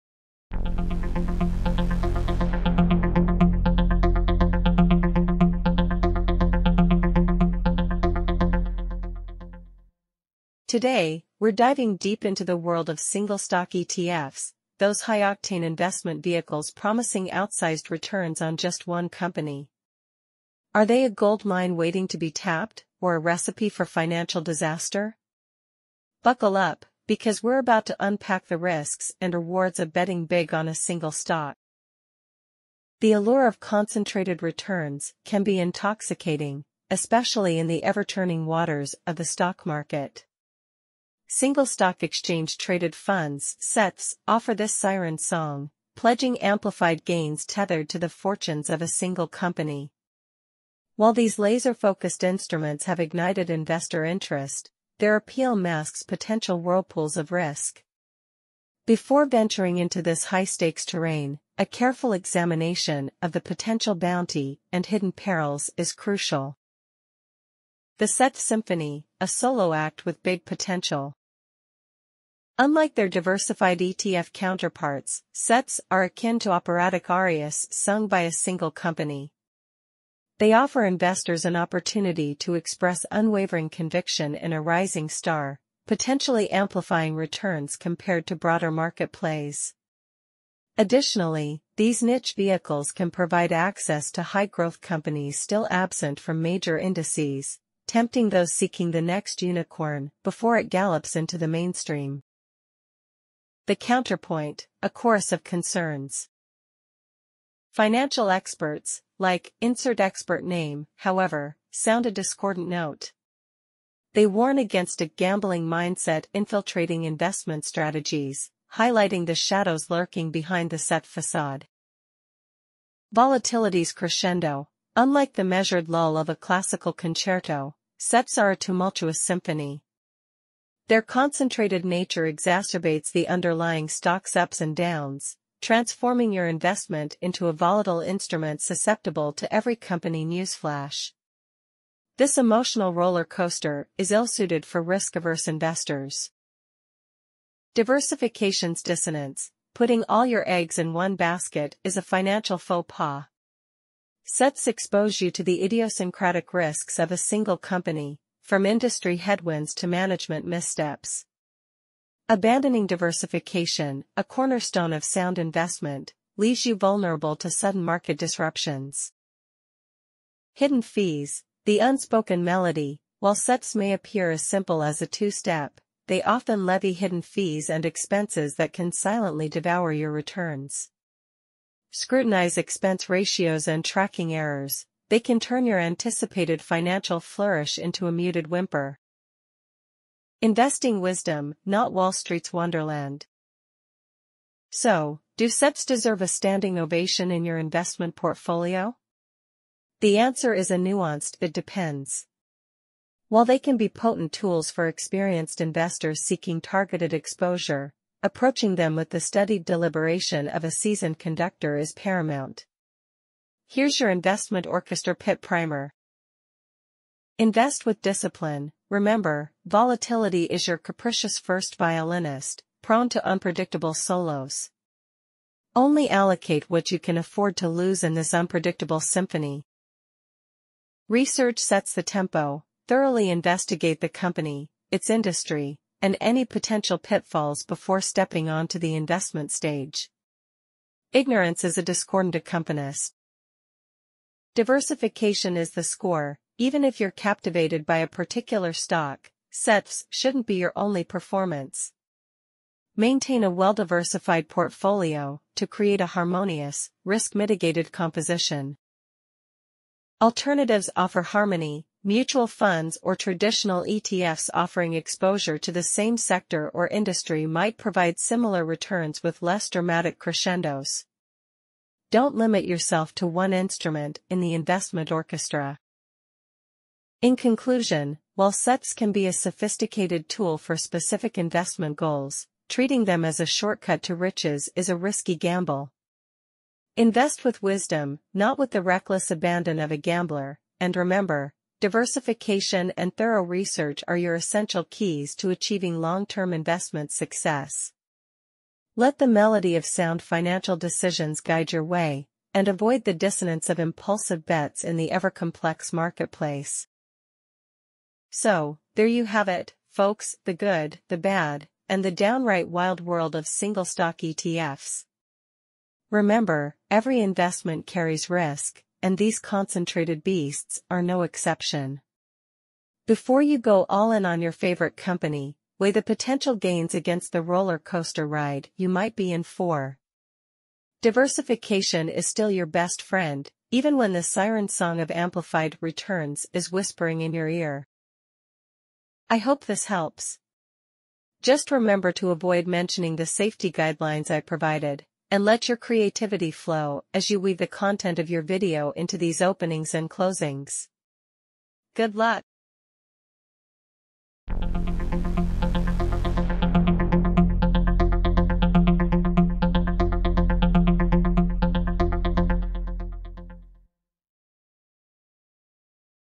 Single Stock ETFs Hot Bet or Risky Gamble
Single-stock ETFs offer targeted exposure to a single company, potentially amplifying returns but also amplifying risk. While their concentrated nature can be tempting for seasoned investors seeking specific gains, their volatility, lack of diversification, and hidden fees make them unsuitable for most. Approaching them with careful research, discipline, and within a diversified portfolio is crucial. Remember, investing in SSETFs should be a deliberate act, not a reckless gamble.